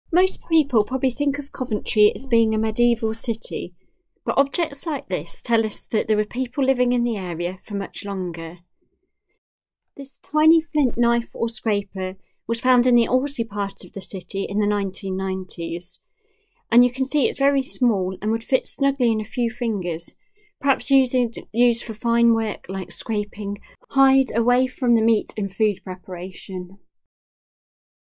Flint Tool found in Coventry
Prehistoric Coventry 😎
This late Neolithic flint knife blade was found in Cheylesmore in 1992.

Let us know what you want to see!